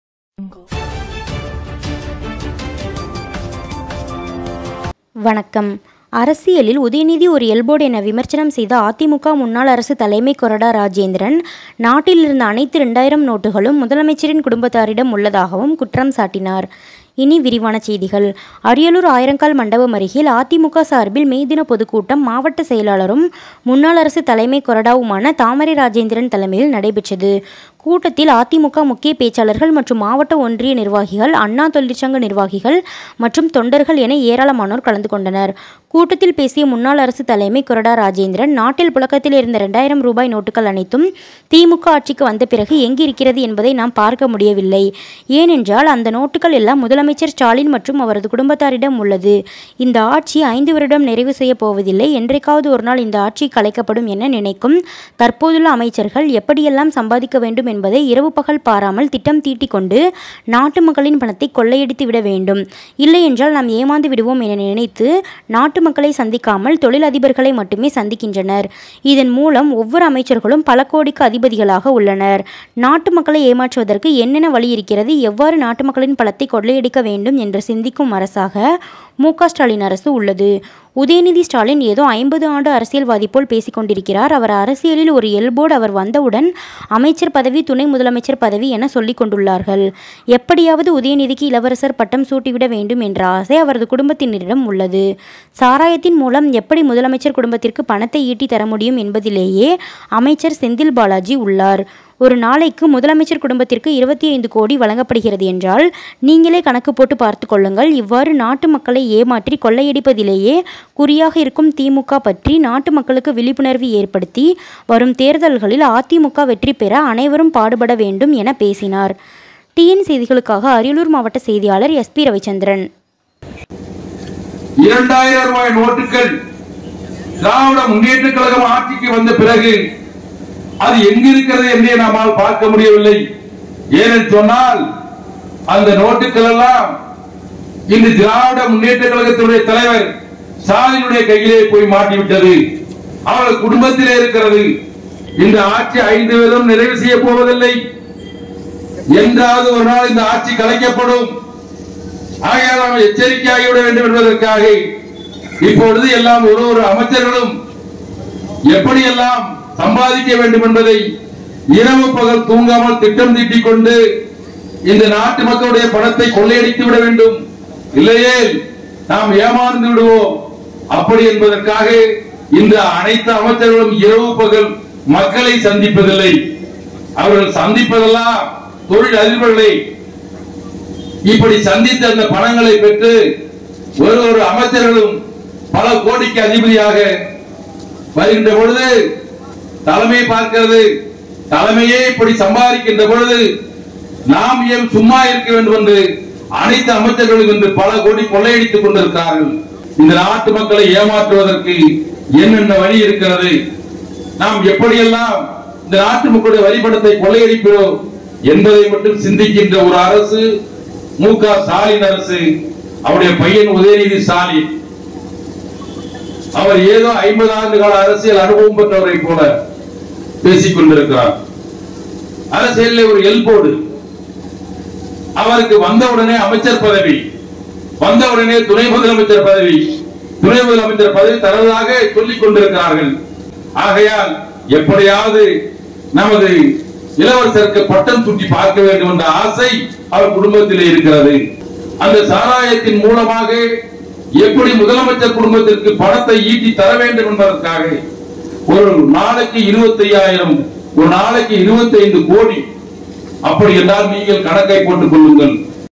உதயநிதி ஸ்டாலின் ஒரு எல் போர்டு 2000 ரூபாய் நோட்டு முதலமைச்சர் இடம் உள்ளது என குற்றச்சாட்டு.
tamilan Ravi
அரியலூர் மாவட்டத்தில் தொழிலாளர் தின பொதுக்கூட்டம் அண்ணா திமுக சார்பில் நடைபெற்றது தலைமை ஏற்று நடத்திய முன்னாள் கொரடா பேசுகையில் உதயநிதி ஸ்டாலின் ஒரு எல் போர்டு நாட்டிலுள்ள 2000 ரூபாய் நோட்டு அனைத்தும் முதலமைச்சரிடம் உள்ளது என்று குற்றச்சாட்டு